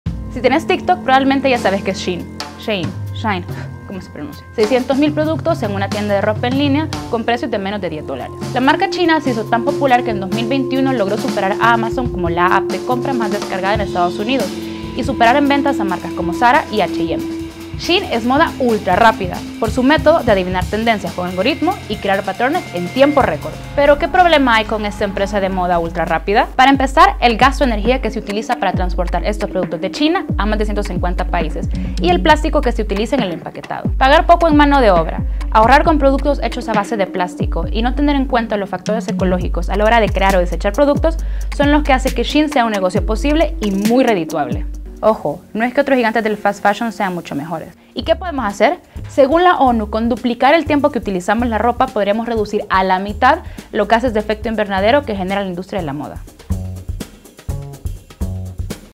¿Cuál es el problema con Shein... y el fast fashion?
Marcas como Shein han creado un sistema de moda ultrarrápida. A pesar de contar con ropa más accesible, el modelo de negocio de estas marcas conlleva consecuencias ambientales graves debido a su modelo de sobreproducción y la velocidad a la que se desactualizan, estropean y desechan sus productos en tendencia.